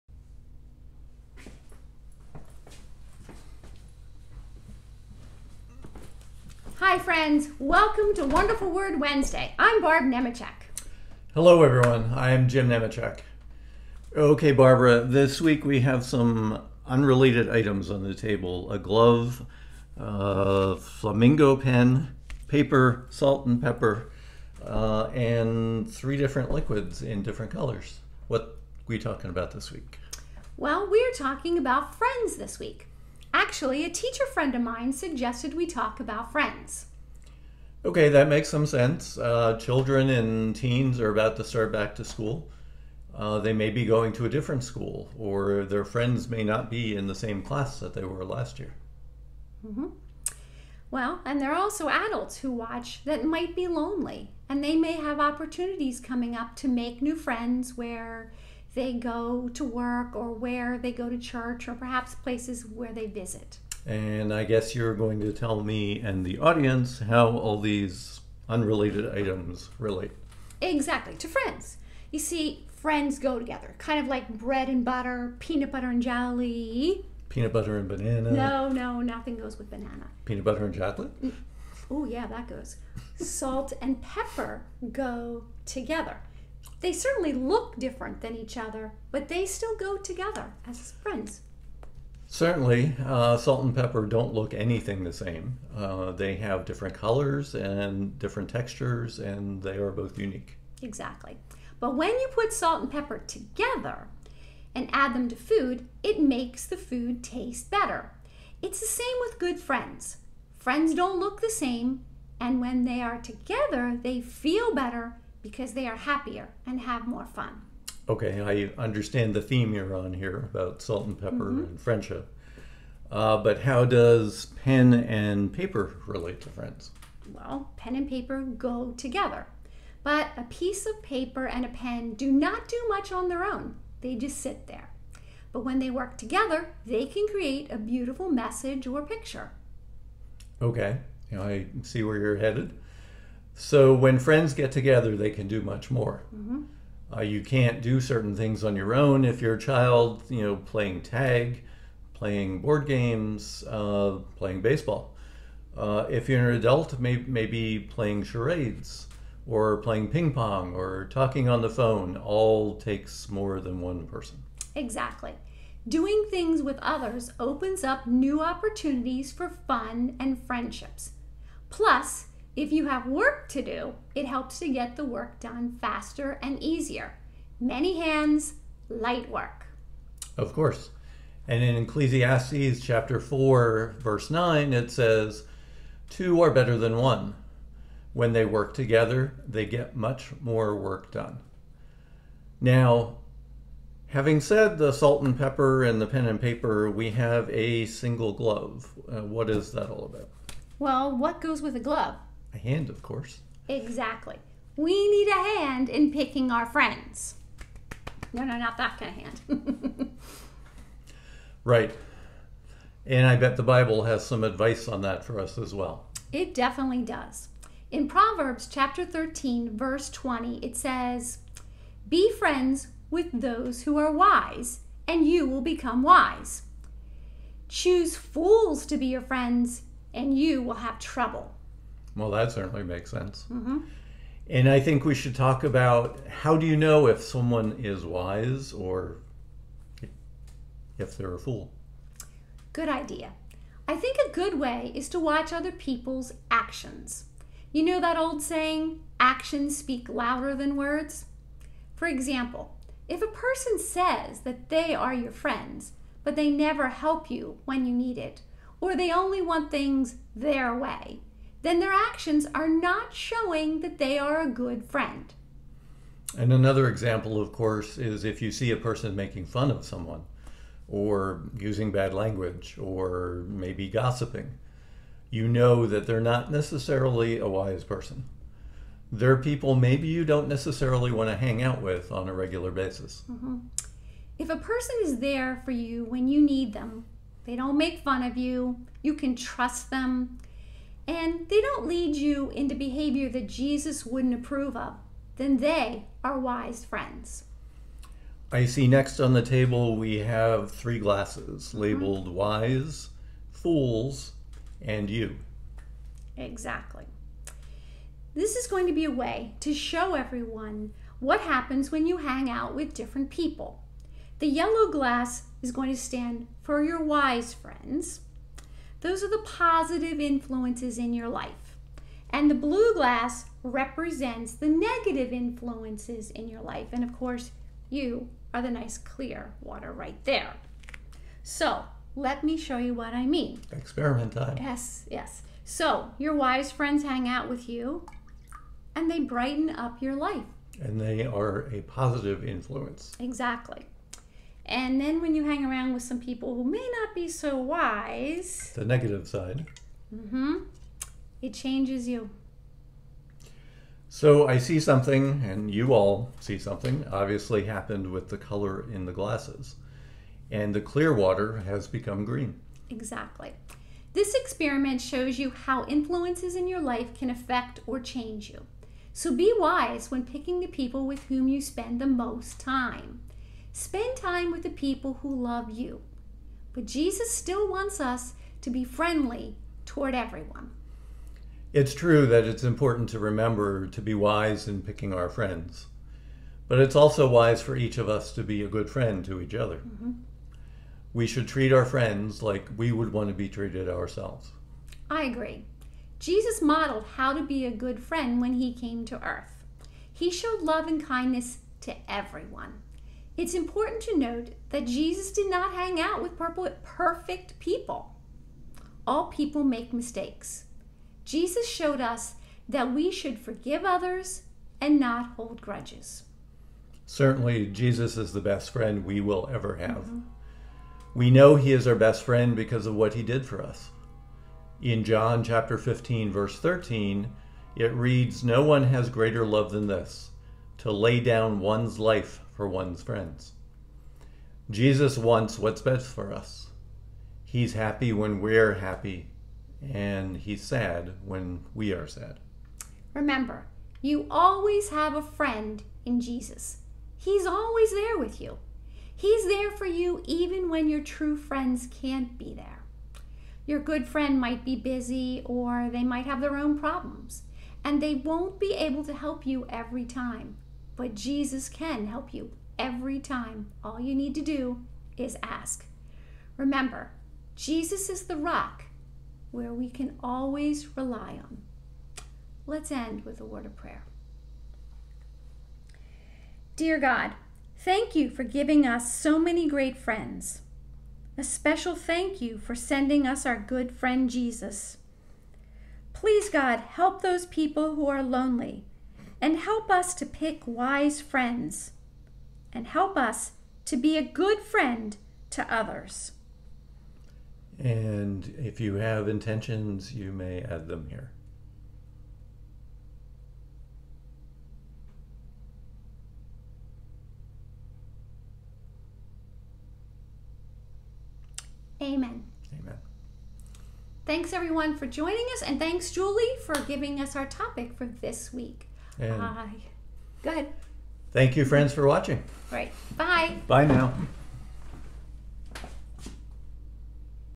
Friends 8 18 21
How do salt & pepper, pen & paper, and a glove relate to friendship?